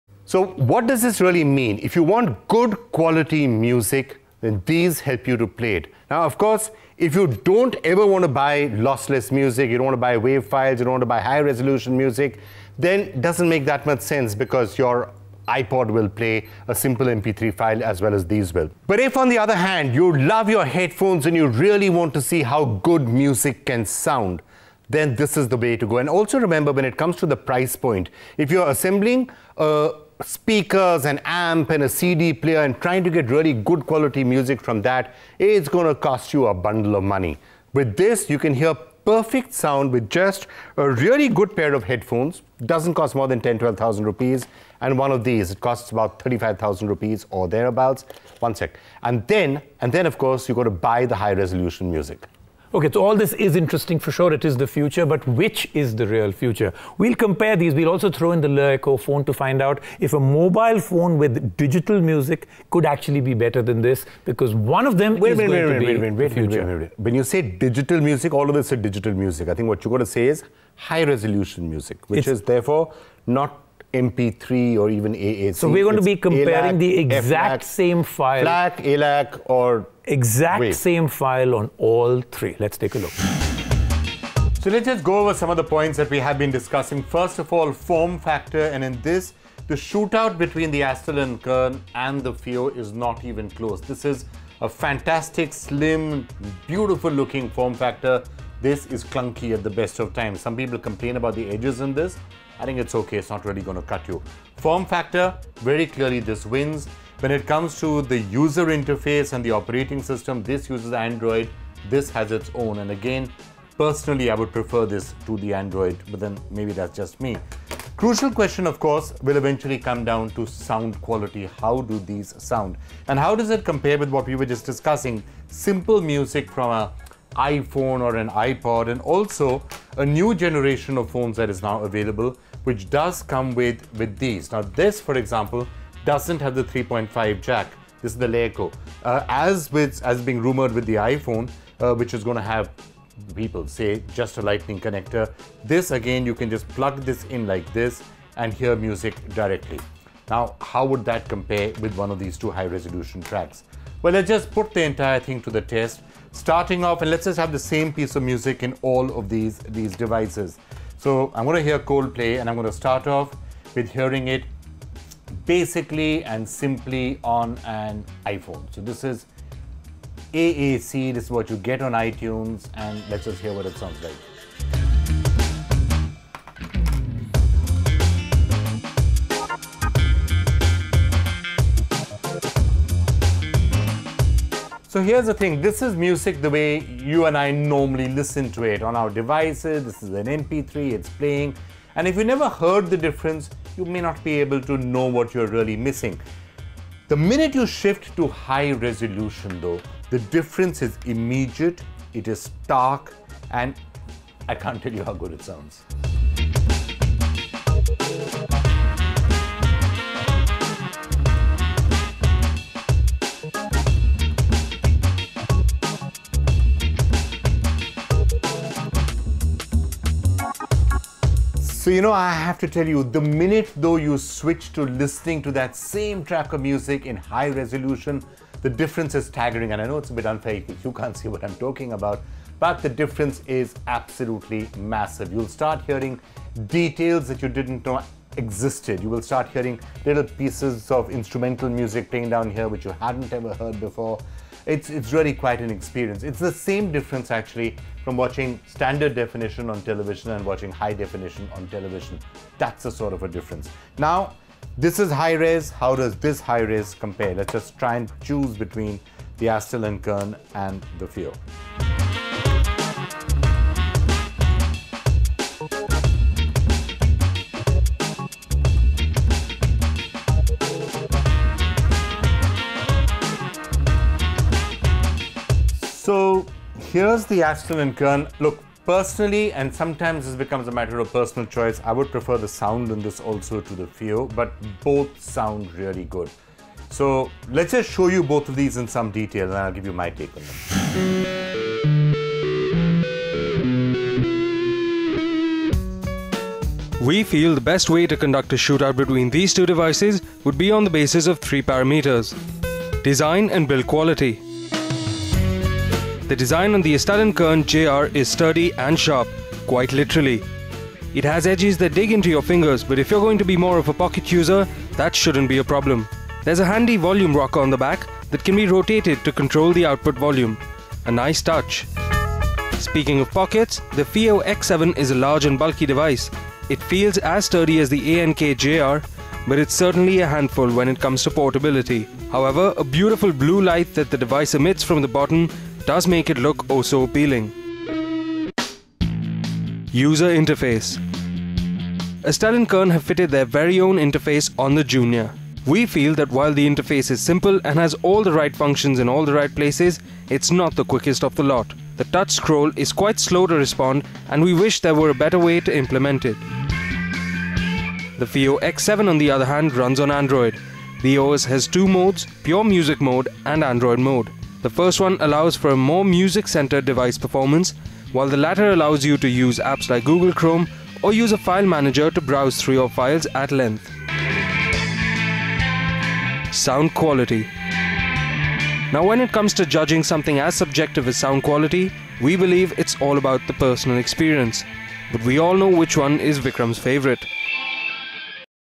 Hi-res audio player shootout
Vikram has always been an audiophile, and this week was no different. He got a hold of two great hi-res audio players: the Astell&Kern JR, and the Fiio X7. He compared them, and then we had a showdown between the devices based on design & build quality, user interface, and sound quality. Take a listen!